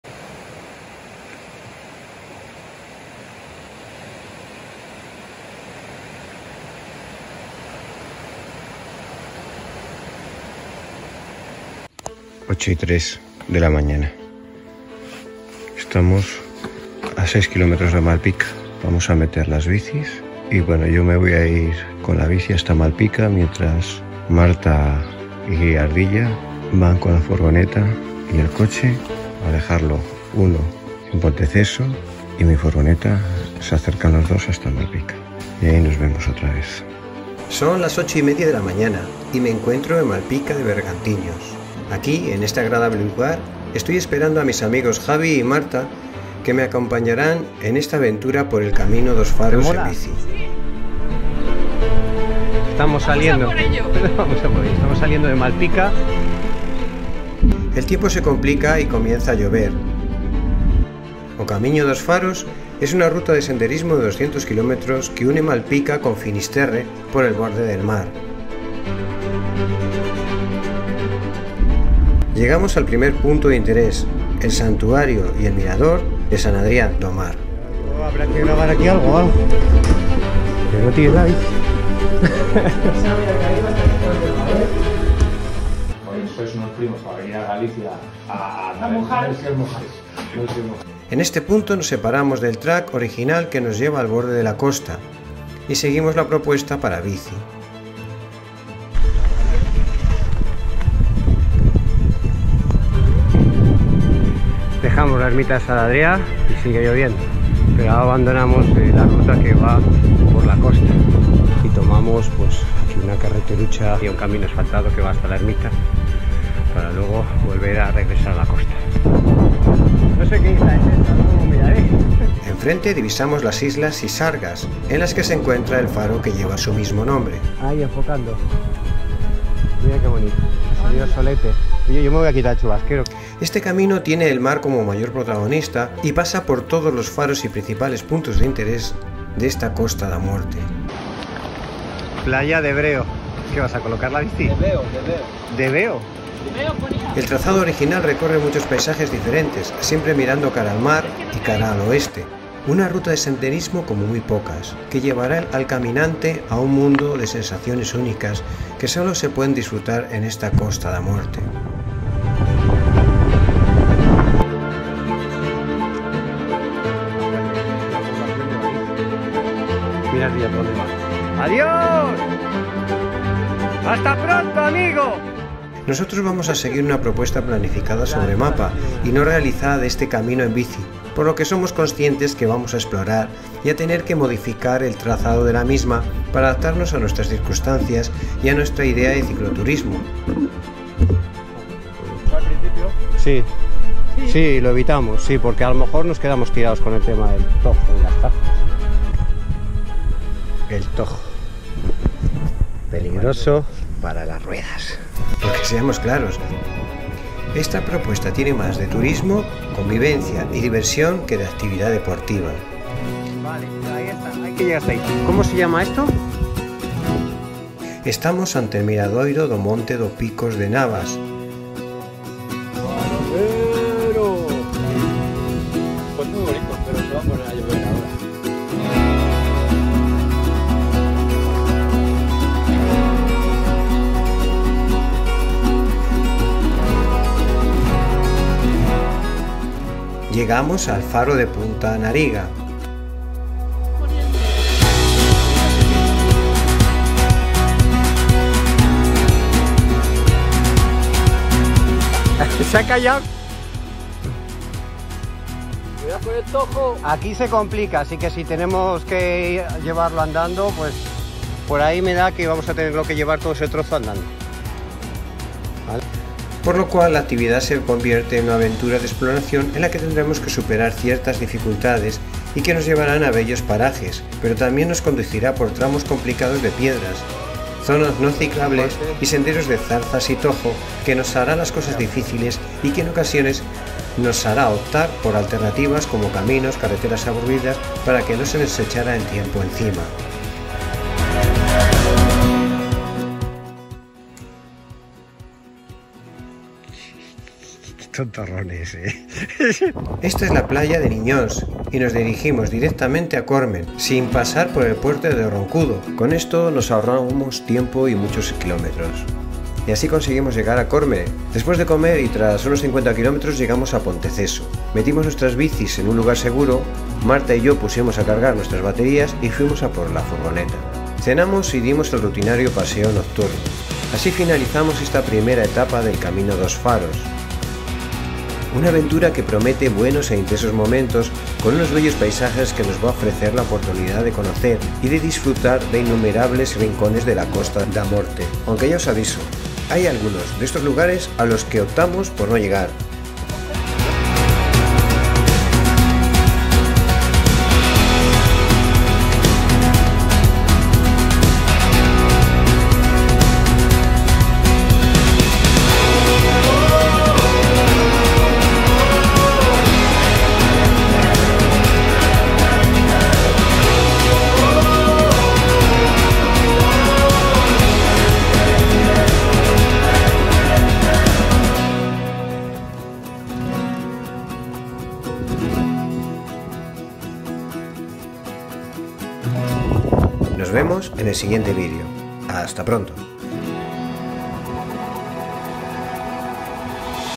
CAMINO DOS FAROS EP.1: De Malpica a Ponteceso
Primera etapa del Camino dos Faros en ebike que realicé junto a mis amigos Javi y Marta.
Seguiremos una propuesta planificada sobre mapa y no realizada de este camino para mtb, por lo que somos conscientes de que vamos a tener  que modificar el trazado de la misma para adaptarlas a nuestras circunstancias  y a nuestra idea de cicloturismo; por que seamos claros, esta aventura tiene más de turismo, exploración, convivencia y diversión que de actividad deportiva.